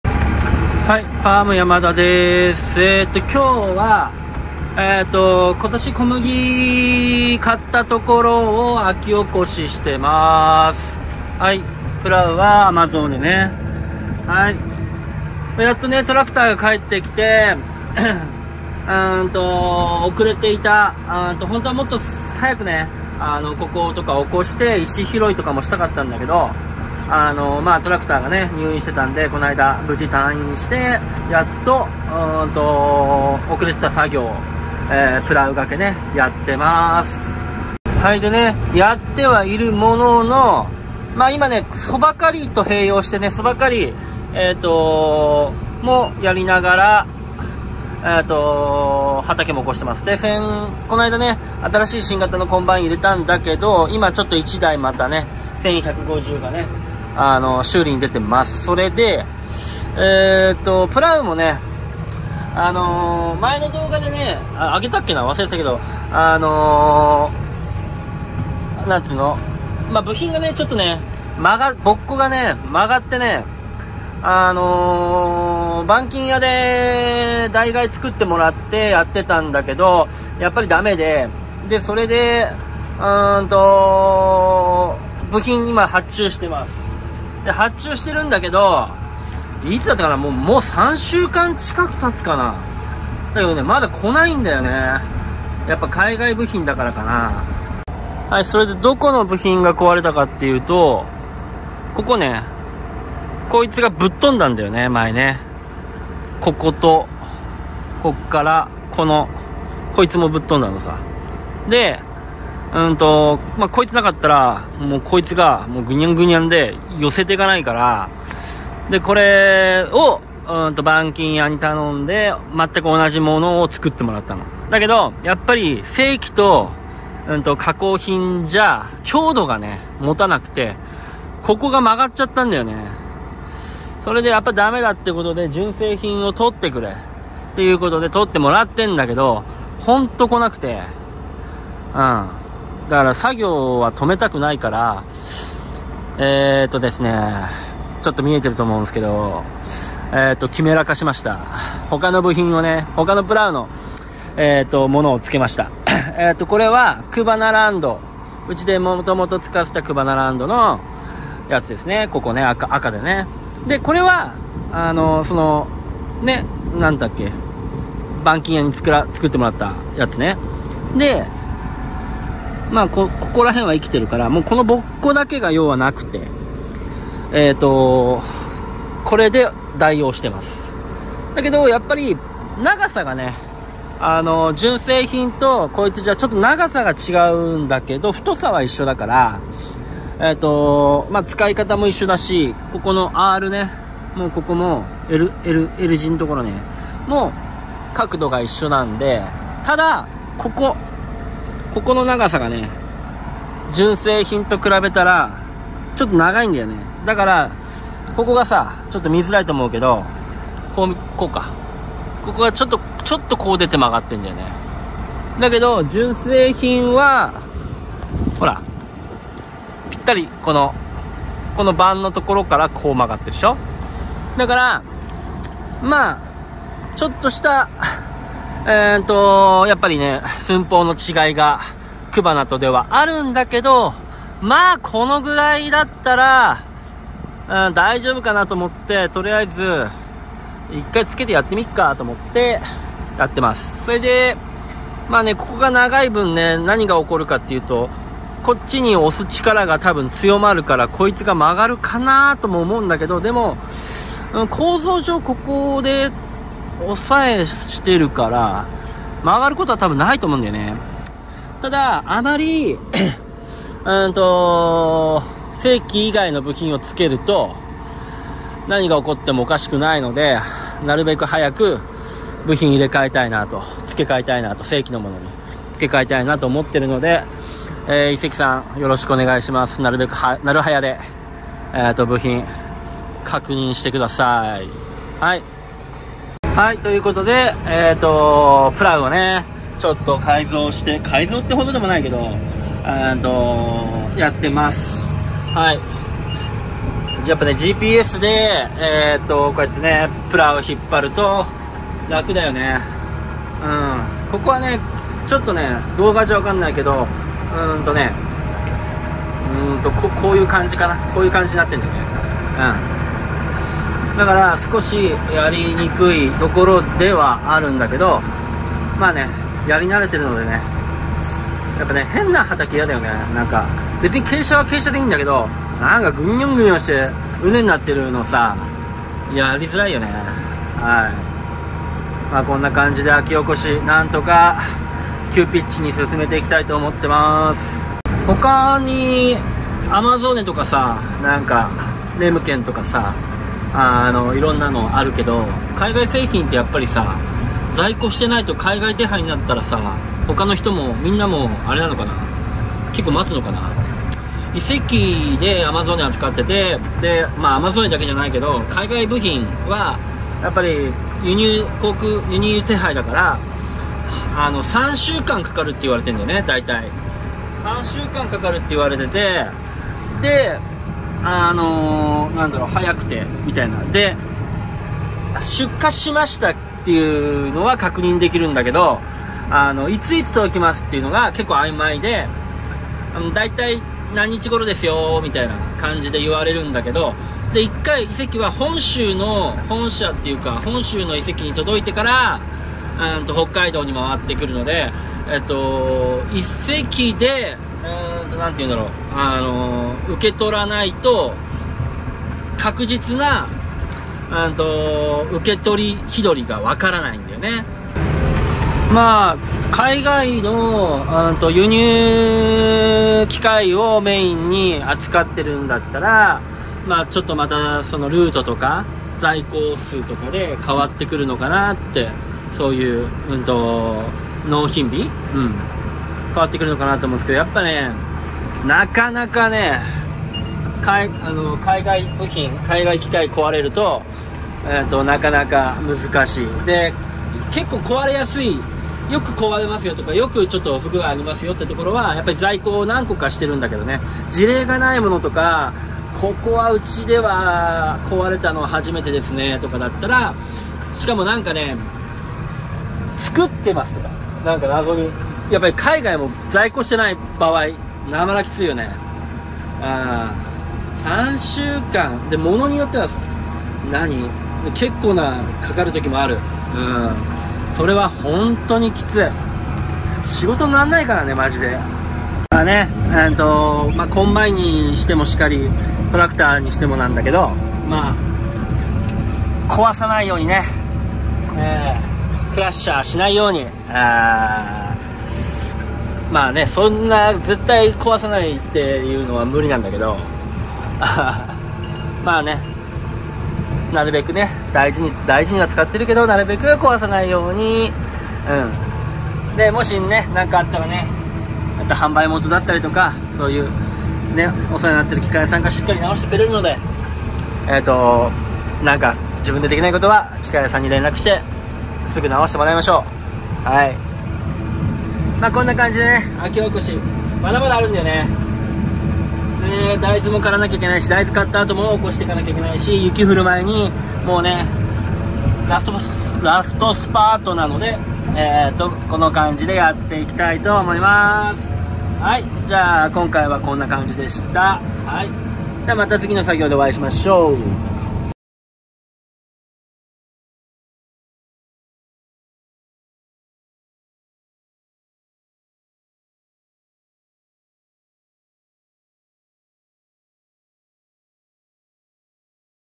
プラウをキメラ化⁉～その名もクバゾーネ～
夏は農業、冬はスノーボードに特化した動画をお届けいたします

プラウの部品が間に合わず急遽キメラ化
2台のプラウで部品取り